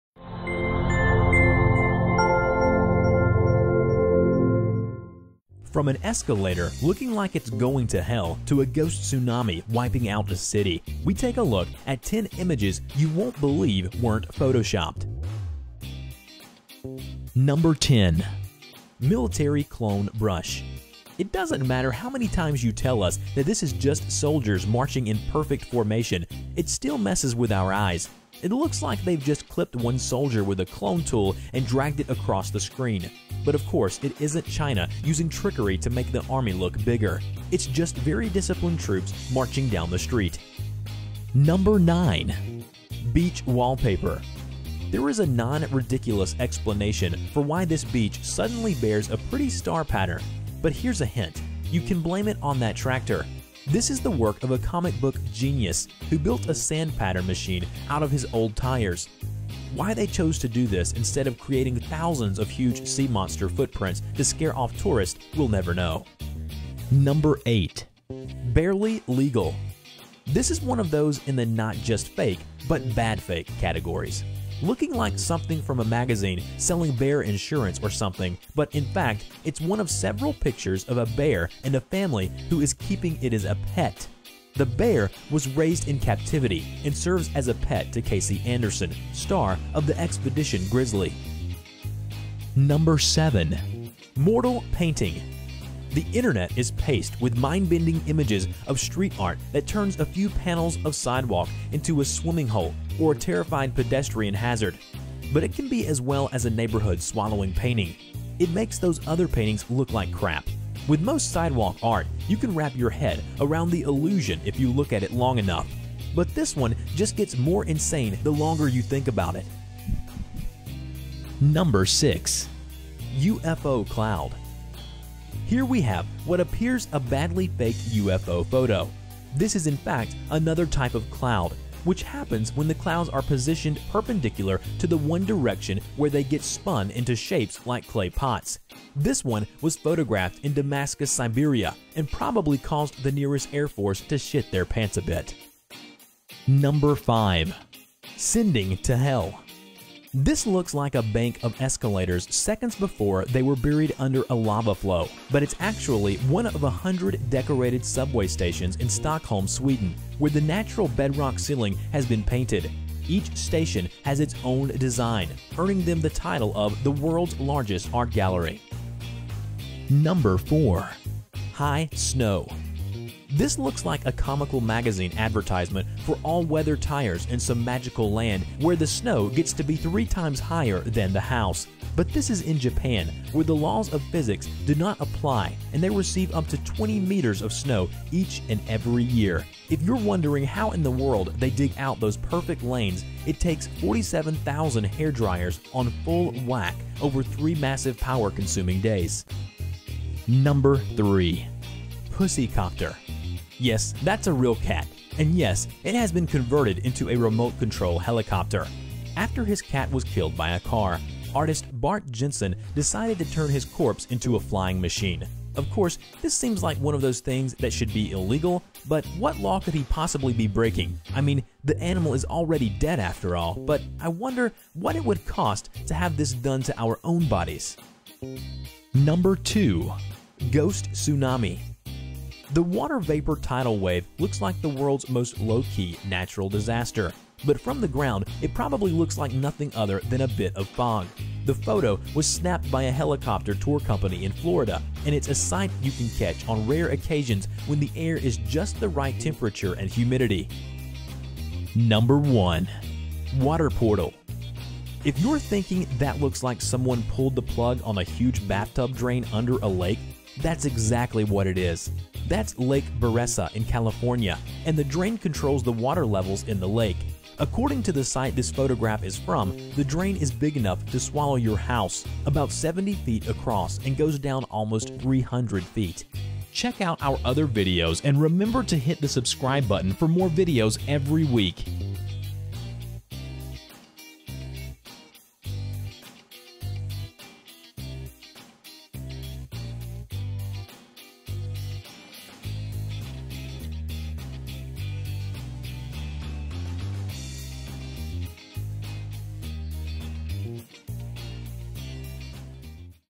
10 PHOTOS You WONT BELIEVE Weren’t Photoshopped | LIST KING
From an escalator looking like it’s going to hell to a ghost tsunami wiping out a city! We take a look at 10 Images You Won’t Believe Weren’t Photoshopped!

Countdown List

10 - Military Clone Brush
9 - Beach Wallpaper
8 - Barely Legal
7 - Mortal Painting
6 - UFO cloud
5 - Sending to Hell
4 - High Snow
3 - Pussy Copter
2 - Ghost Tsunami
1 - Water Portal

Legality Agreement

Background music copyright Jason Shaw (audionautix.com)
Unported License.